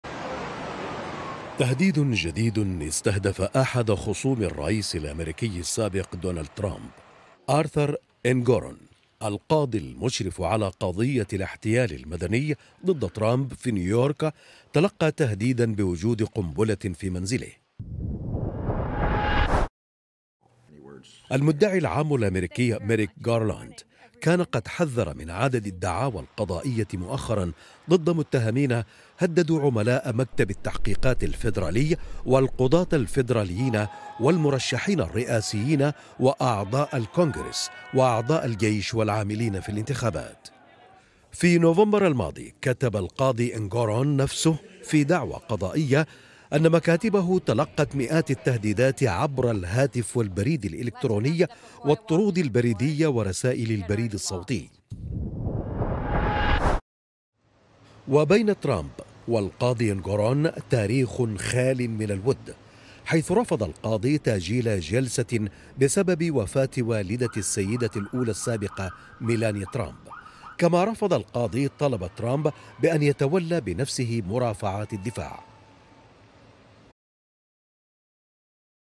ترمب ينتظر حكما من قاض بينهما تاريخ خال من الود
محاكمة ترمب بتهم الاحتيال تقترب من مرحلة إصدار الحكم.. وأنباء عن تهديدات للقاضي